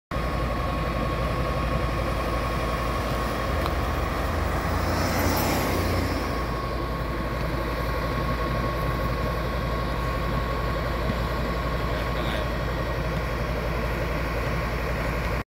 SCDF PL231 PL232 CP221 ( TAMPINES FIRE 07AUG2021 )
Blk 823A Tampines St 81
Coffee Shop on Fire on 07Aug2021
Attending appliances from Tampines Fire Station with Changi Fire Station
PL231 PL232 A231
PL222 CPL221